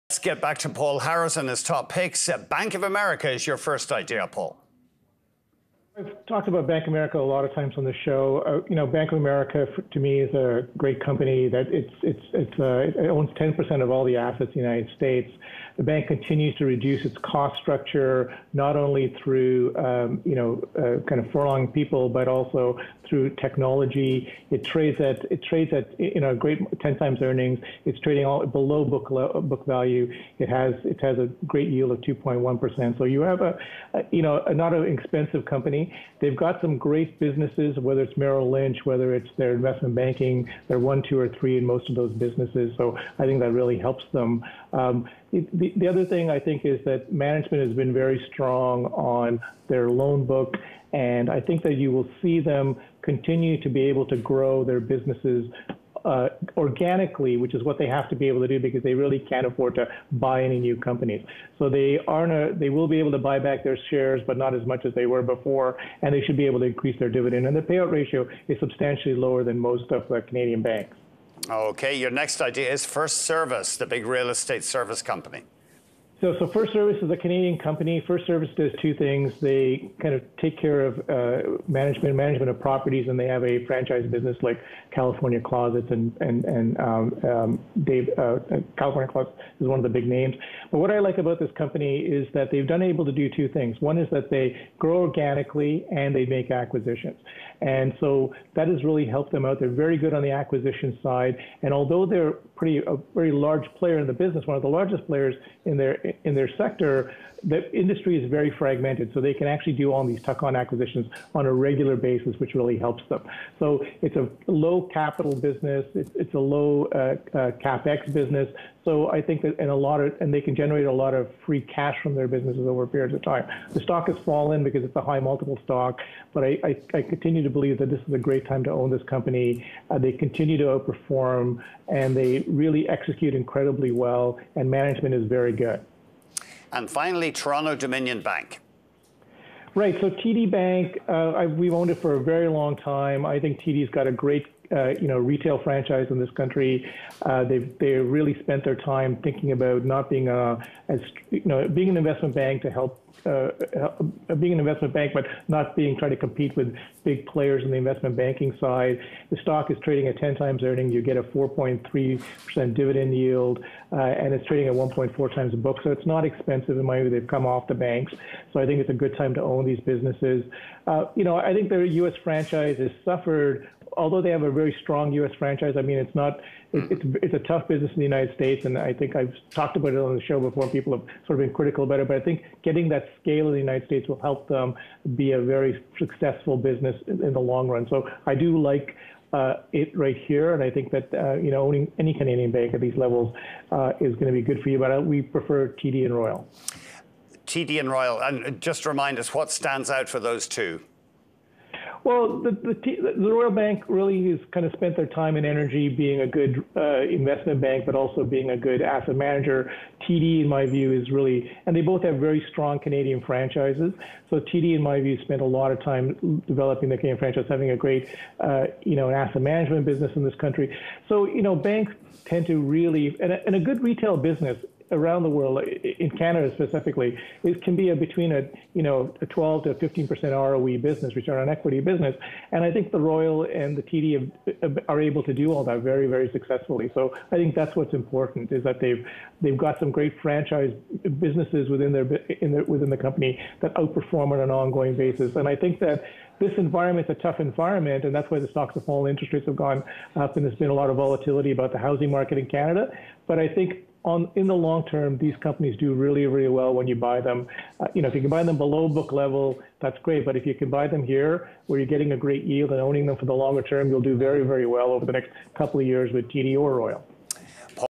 Paul Harris' Top Market Picks
Paul Harris, Partner and Portfolio Manager, Harris Douglas Asset Management FOCUS: North American and Global Stocks.